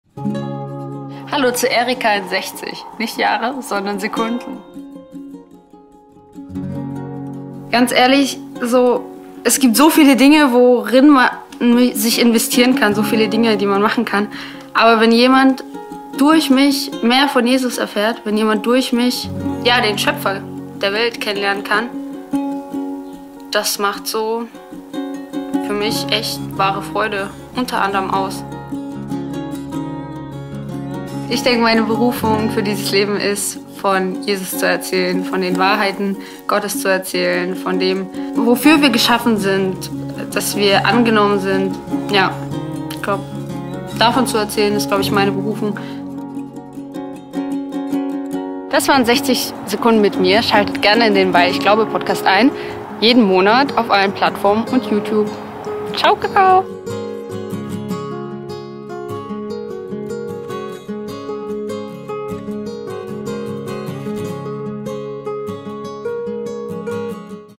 Feature: Erika in 60 Sekunden
Lerne mit uns gemeinsam Erika kennen. Was treibt sie an, den »Weil ich glaube« Podcast zu machen und was ist ihr im Leben wichtig? All das in 60 Sekunden.

Erikas Podcast erscheint monatlich neu hier auf YouTube, Spotify & Apple Podcast.